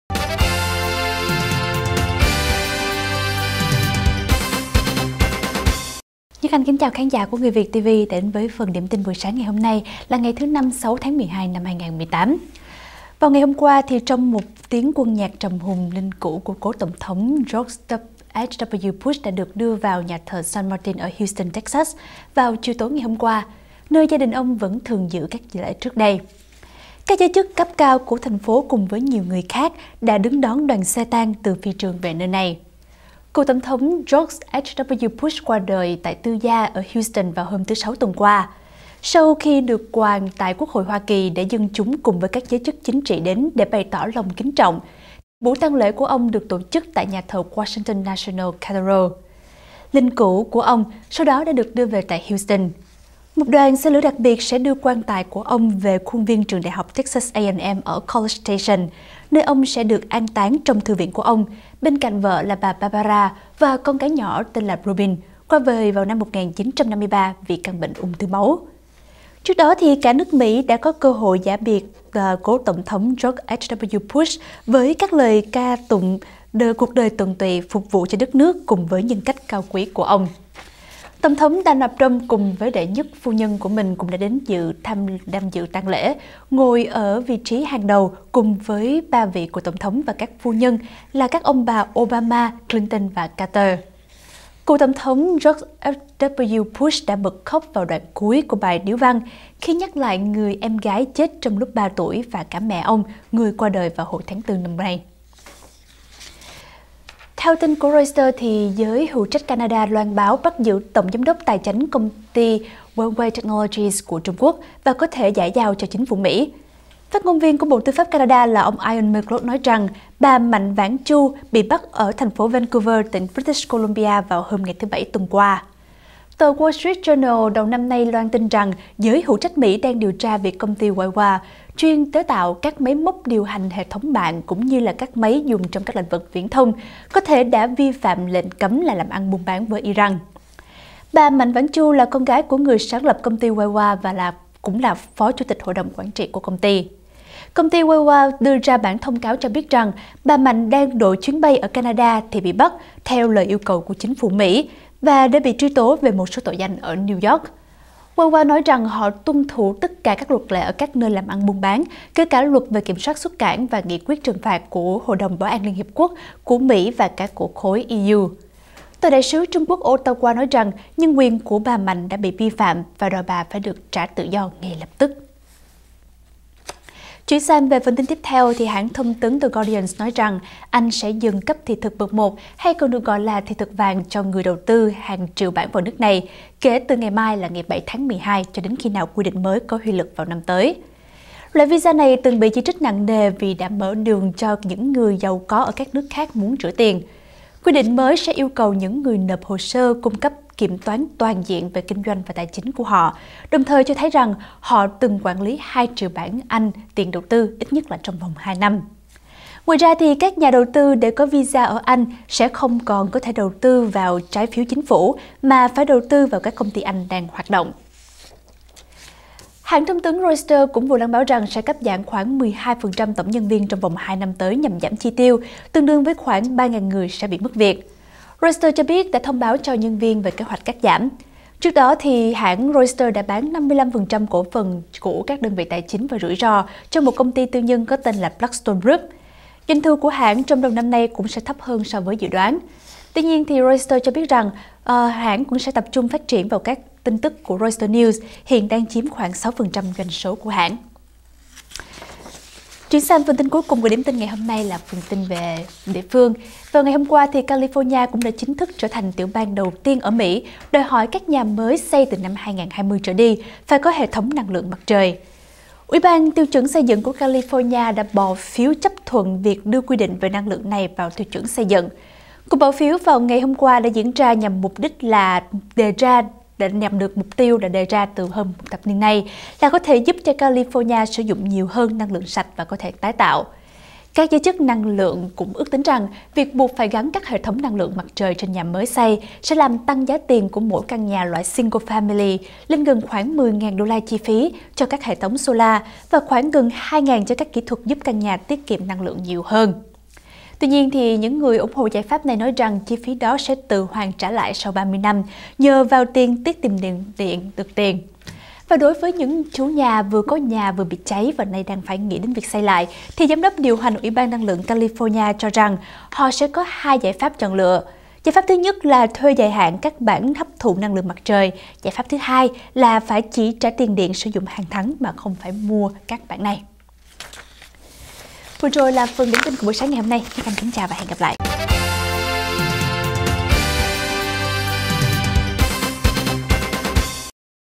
Điểm tin buổi sáng ngày 6 tháng 12 năm 2018
-Cố TT George H.W. Bush về lại Houston, đến nơi an nghỉ sau cùng
-Canada bắt giám đốc tài chánh Huawei Technologies, chờ giao cho Mỹ
-Anh ngừng cấp ‘thị thực vàng’ cho giới đầu tư và giới định cư
-Reuters chuẩn bị cắt giảm hơn 3,000 nhân viên
-California buộc nhà mới phải có hệ thống năng lượng mặt trời